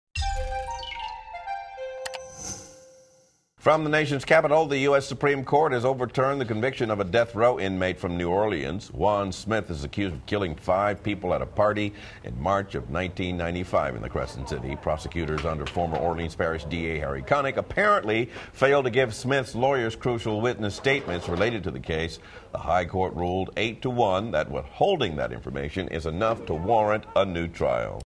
Orleans DA To File Motion After Supreme Court Reverses Conviction
The U.S. Supreme Court has overturned a death row inmate's conviction of killing five people in the justices' latest look at the conduct of prosecutors in the New Orleans district attorney's office.